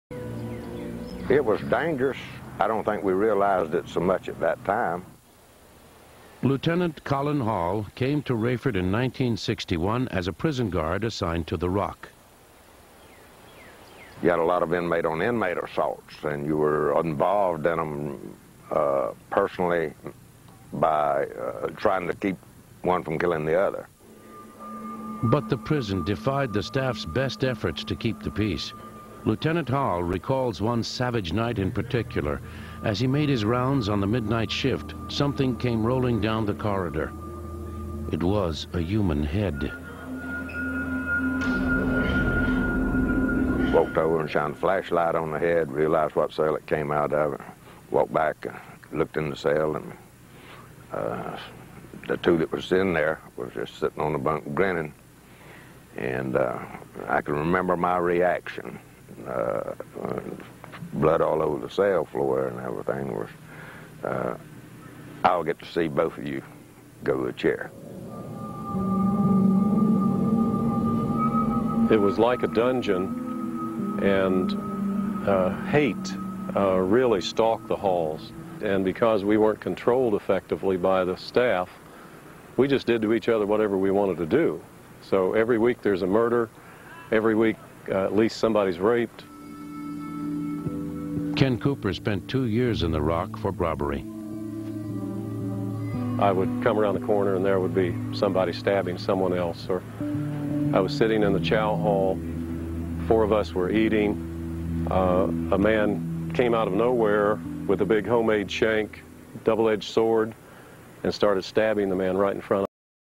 Ken Cooper Talks About The Rock
Ken Cooper, the Gentleman Bank Robber, shares a little about his time at The Rock - considered one of the worst prisons in the USA at the time.  for more information about Ken and his life.